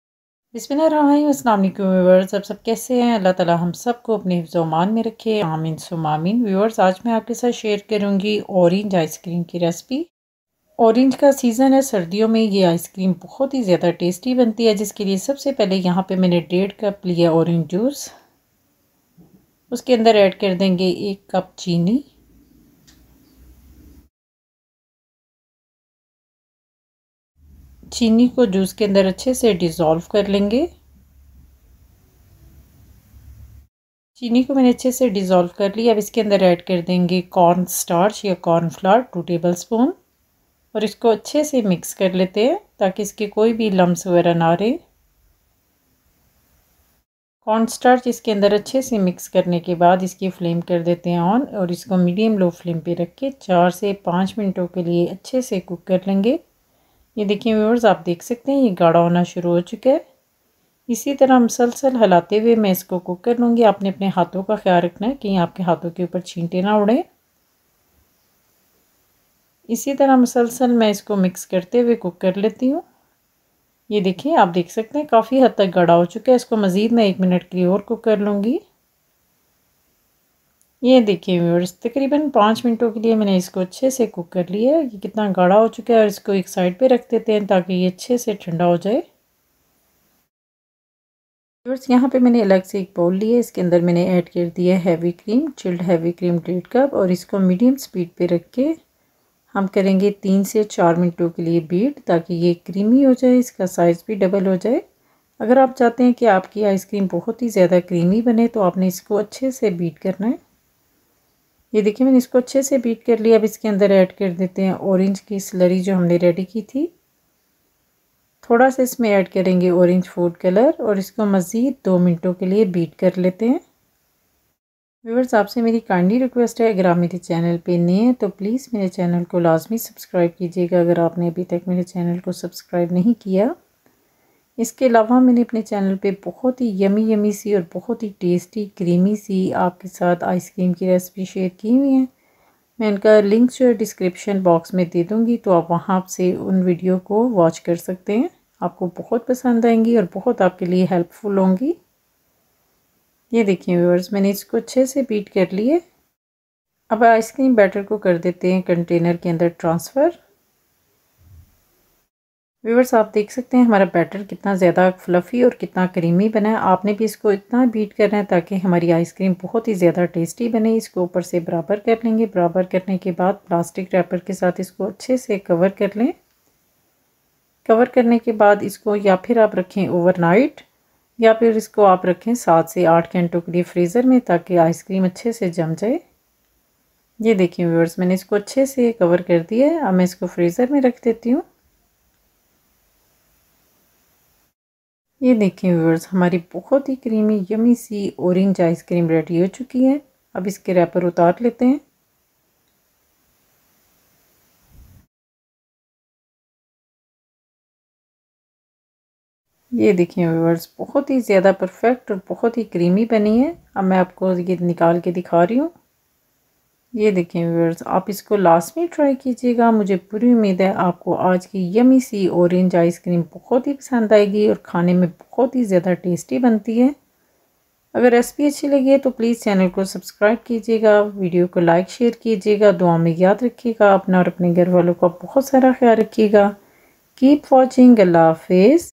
Orange 🍊 Ice Cream Recipe.No Condensed milk, No Ice cream maker,No Artificial flavor
tasty cooking with Tabassam
Ingredients:
orange juice 1&1/2cups
sugar 1 cup
heavy cream 1&1/2cups

Mango Custard Ice cream

1 Minute Sugar Free Ice Cream

Pineapple Ice Cream Swiss Roll

Kulfa Ice cream recipe

Chocolate Chip Vanilla Ice cream